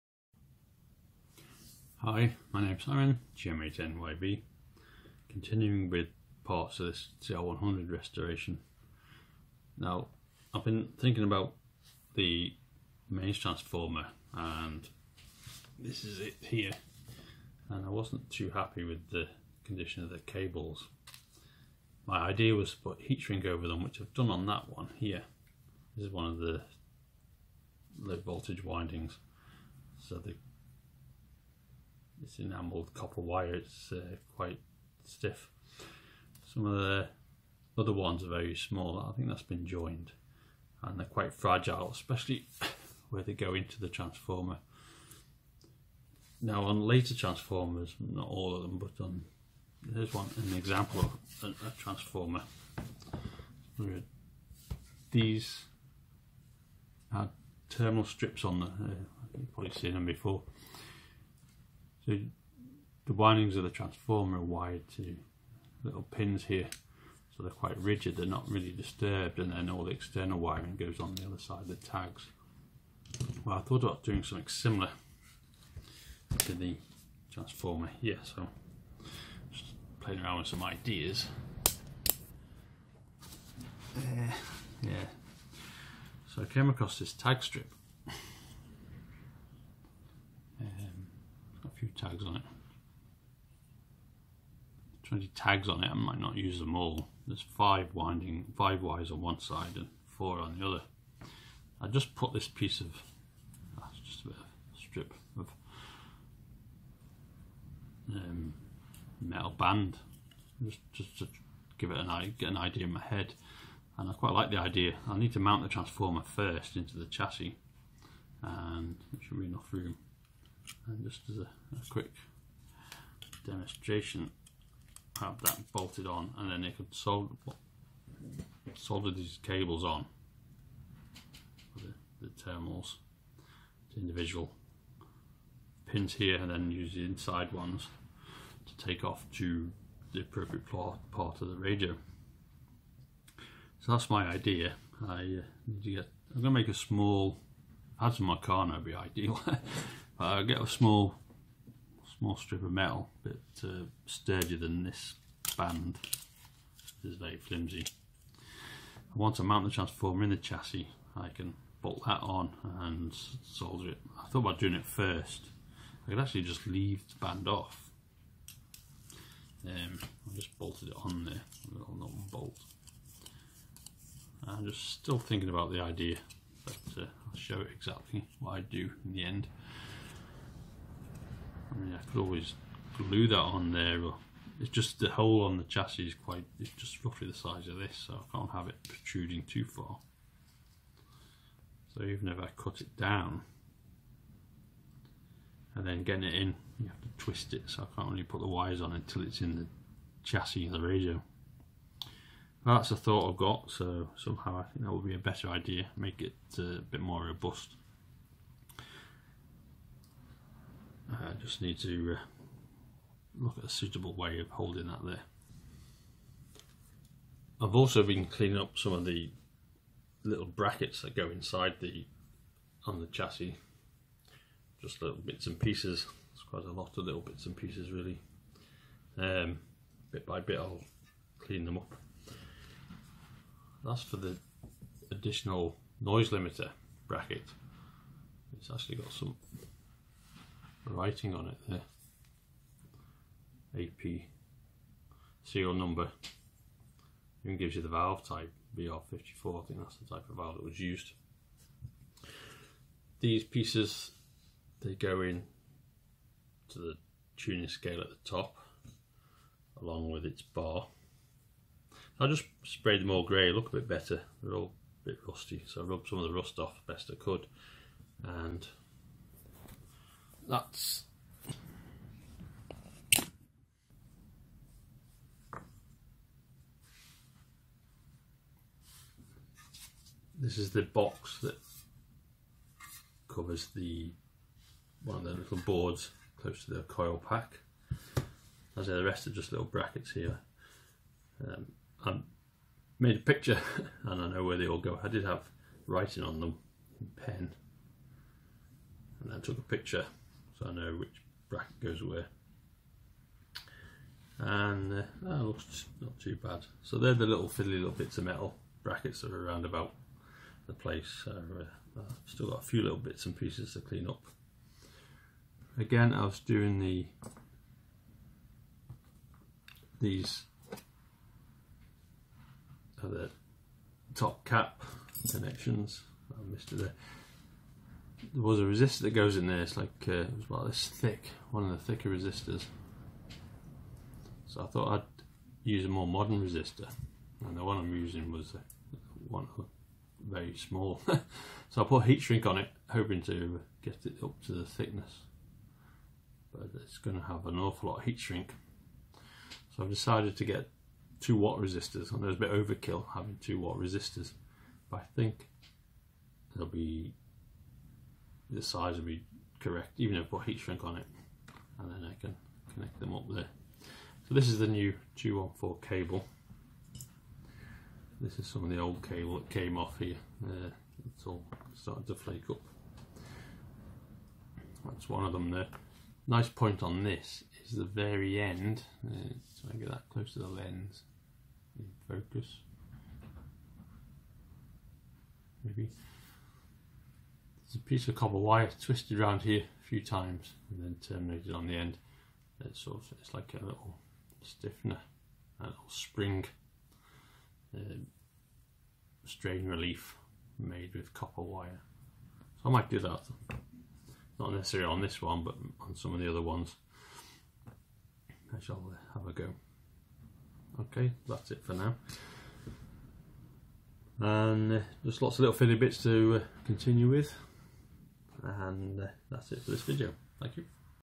Marconi CR100 Renovation (23) Transformer ideas,
I discus a method of connecting the fragile transformer wiring.  A look at some of the small brackets.